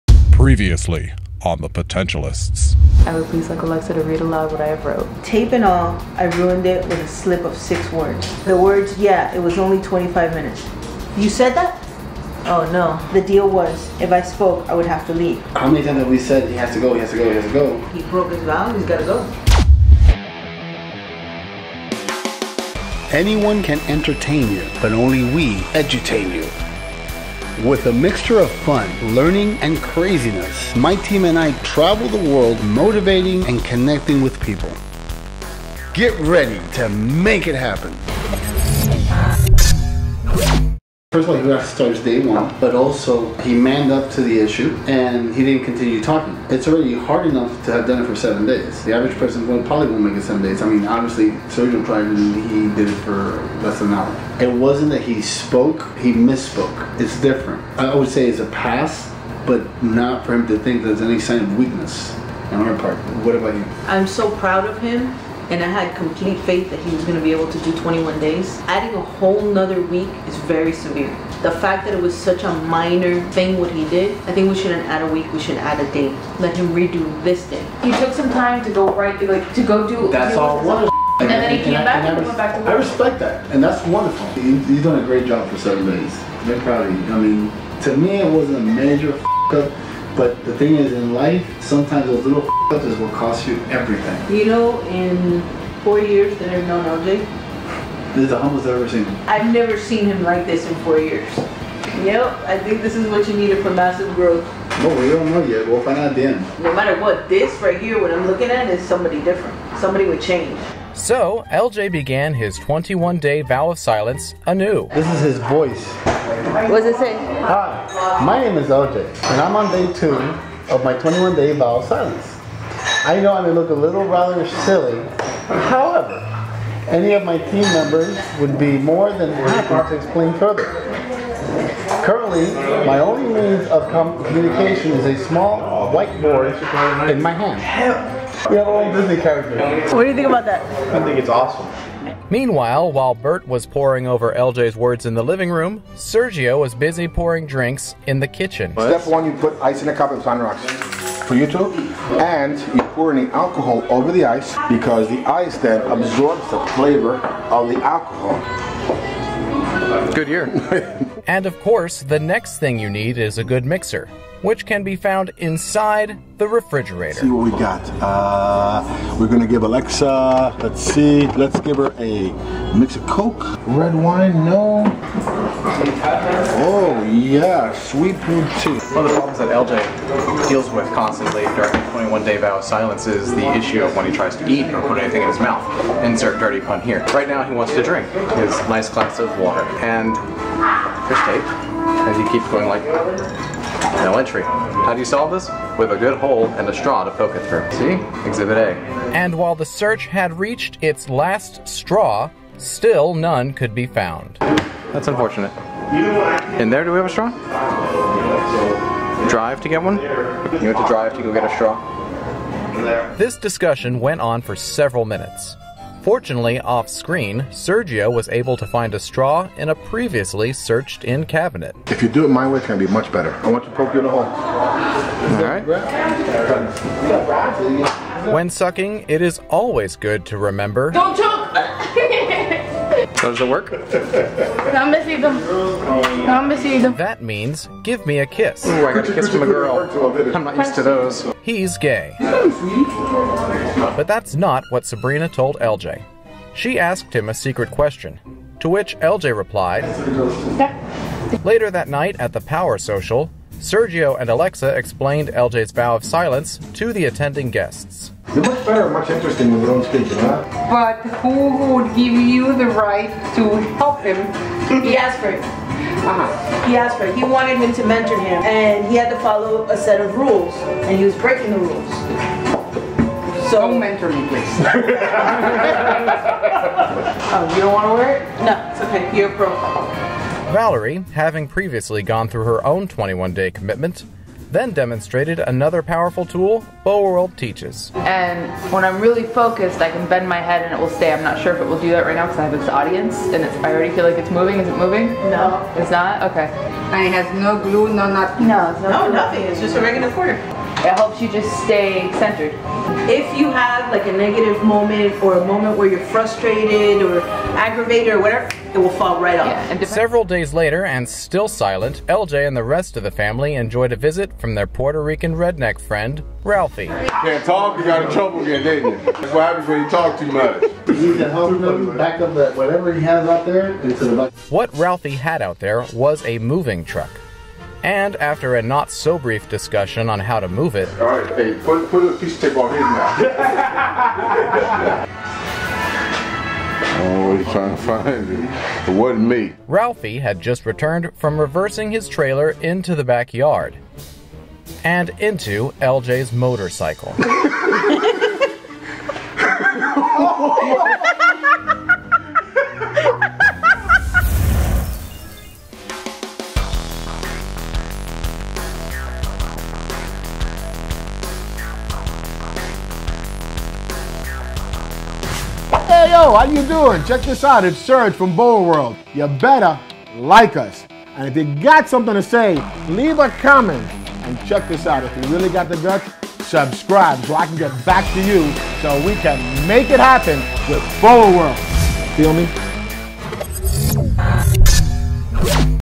VOW OF SILENCE (Season 1, Episode 91)
Who is committed to making a change for personal growth? Who drew the last straw? Who was involved in a motorcycle accident? Watch to find out!

The Stunt Vlogger recommits himself to the twenty-one day vow of silence after a slipup. During the ensuing days, the Motivation Mogul, Marketing Crusader and Master Handshaker explain the Stunt Vlogger's commitment to guests attending their Power Social event. Then, a visit from a family friend tests the Stunt Vlogger's vow of silence in a BIG way.

Bert Oliva
12080 Southwest 127 Avenue
Suite 305